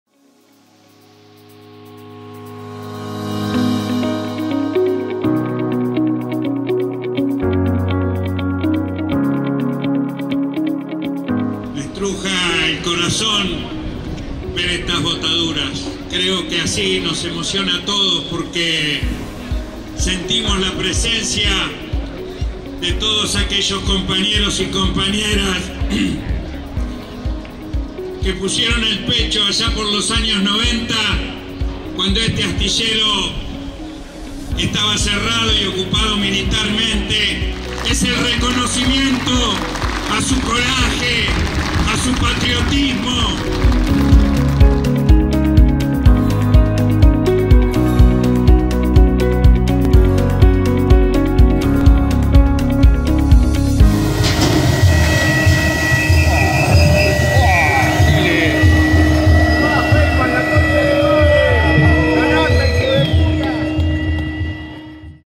Astillero Río Santiago: Botadura histórica
Redacción Canal Abierto | Se realizó la botadura de la compuerta que construyó el Astillero Río Santiago para el dique 2 del Arsenal Naval Puerto Belgrano de la Armada Argentina, una pieza que se hizo íntegramente en la planta naval ubicada en Ensenada.

Se trata de una mole de más de ochocientas toneladas de acero naval que mide aproximadamente cuarenta metros de largo por treinta y seis de ancho. Fue construida por casi novecientos trabajadores durante más de un año.

Participaron del acto el jefe de Gabinete y precandidato a vicepresidente de Unión por la Patria (UxP), Agustín Rossi; el ministro de Defensa, Jorge Taiana; el gobernador Axel Kicillof; el titular de ATE Nacional y CTA Autónoma, Hugo Godoy; el titular de ATE provincia de Buenos Aires, Oscar de Isasi, y el de Ensenada, Francisco Banegas; la presidenta del Correo Argentino, Vanesa Piesxiorovski; los intendentes de Berisso, Fabián Cagliardi, y Ensenada Mario Secco; y el ministro de Producción, Augusto Costa, entre otros.

«Le tapamos la boca a los agoreros de la derecha. Teníamos razón. Hicimos la compuerta. El Astillero se puso en marcha y produce», apuntó Kicillof.

El gobernador de la provincia de Buenos Aires además anunció que “después de haber logrado que en el Presupuesto Nacional se modifique una normativa, en el día de ayer, todo el área de gobierno que depende de Sergio Massa, ministro de Economía y futuro Presidente de todos los argentinos, empujó por un decreto que lleva la firma del Presidente por el cual, a partir de ahora, nuestro Astillero puede colocar producción en el mercado interno. Mientras algunos trabajan para cerrar esta empresa estatal otros trabajamos para darle más fuerza y más producción para fortalecer la industria nacional, la defensa, la soberanía y lo que es nuestro”.

Por su parte, el secretario General de la Asociación Trabajadores del Estado, Hugo Godoy celebró la botadura: «Seguimos trabajando por más soberanía y Justicia Social».

“Sentimos la presencia de todos aquellos compañeros y compañeras que pusieron el pecho en los años 90 cuando este Astillero estaba cerrado y ocupado militarmente y en la madrugada de un lunes vinimos a abrir un Astillero”, agregó el dirigente estatal.

“Días atrás nos comprometimos en Brasil con el sindicato de Lula, con la CUT y las centrales sindicales de Argentina para que la industria naval, la energía nuclear y aeroespacial sea prioridad en la tarea de los trabajadores y trabajadoras”, contó Godoy.

A su turno, el titular de ATE bonaerense Oscar de Isasi manifestó que “esto representa la síntesis de todo un proceso que marca, no sólo la lucha de los trabajadores y trabajadoras de este Astillero, sino también un horizonte del modelo de sociedad que tenemos que construir”.

“Quiero reconocer al gobernador de la provincia de Buenos Aires, Axel Kicillof, que tuvo la actitud patriótica, allá en Rosario, cuando se planificaba volver a concesionar la vía troncal del Paraná y plantó banderas y dijo que no hay que volver a privatizar y que hay que recuperar la vía troncal de navegación para el Estado nacional y para los argentinos y las argentinas”, agregó.

En tanto, Francisco Banegas, secretario General de ATE Ensenada, destacó: “Esta es la respuesta para aquellos enemigos del Astillero que nos atacan constantemente. A ellos les decimos que al Astillero lo mantuvimos abierto luchando en la calle pero ahora, con las decisiones políticas que está tomando nuestro gobierno, lo vamos a defender con la producción”.

En un comunicado, se indicó que la construcción de esta compuerta -que es la obra de metalmecánica más grande que se está realizando actualmente en la Argentina- «se hizo en el marco de una relación de colaboración entre los astilleros públicos, con el objetivo de potenciar las capacidades tanto de Tandanor como de Astillero Río Santiago para fortalecer nuestra soberanía».

La jornada también contó con el descubrimiento de un mural de Juan Perón ya que el 15 de junio se cumplieron 70 años del decreto presidencial que dispuso la creación de Astilleros y Fábricas Navales del Estado (AFNE). También se bautizó al edificio destinado a la Armada Argentina con el nombre de “Capitán Enrique Carranza”, dos veces presidente del Astillero Río Santiago.